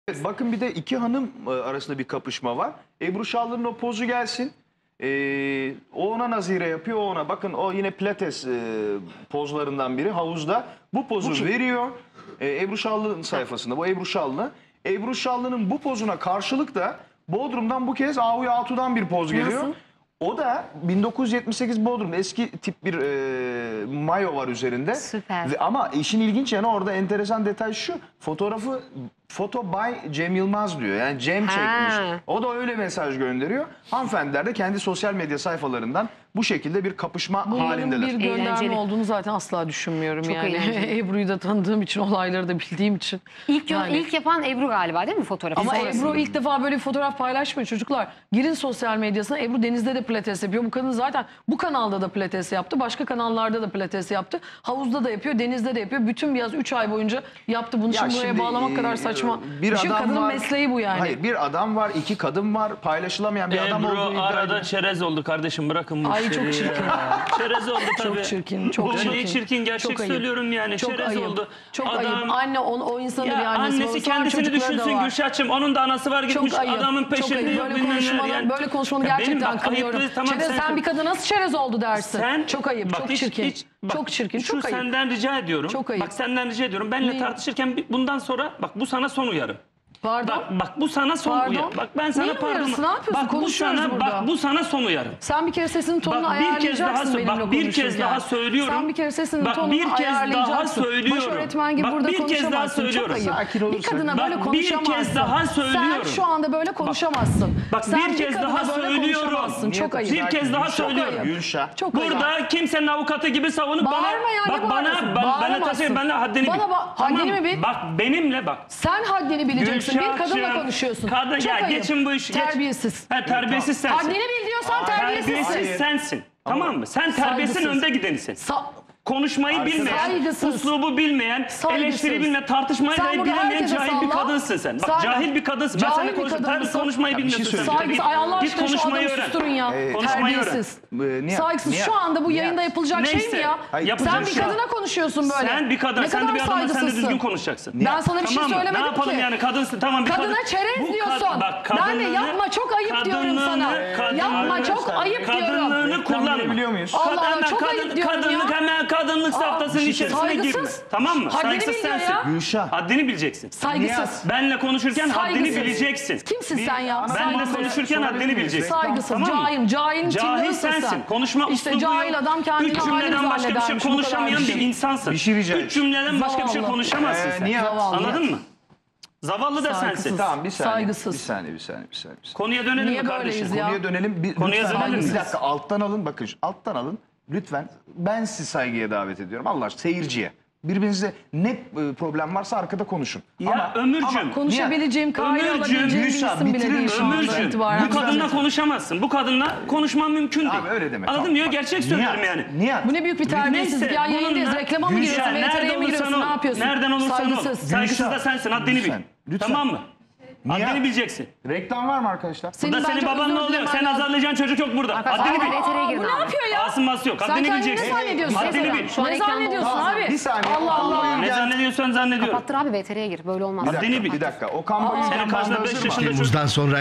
Nihat Doğan'la Gülşah Saraçoğlu canlı yayında fena kapıştı!
Canlı yayında yorumcularımız Nihat Doğan ile Gülşah Saraçoğlu büyük tartışma yaşadı. Nihat Doğan'un Ebru Şallı hakkında ''Çereze döndü'' yorumu Gülşah Saraçoğlu'nun hiç hoşuna gitmedi ve saygıya davet etti. Gülşah'ın sözlerine bağıracak cevap veren Nihat Doğan iyice ortamı gerdi. Program moderatörü Ömür Varol tanıtım veteresi girerek ikiliyi yatıştırdı.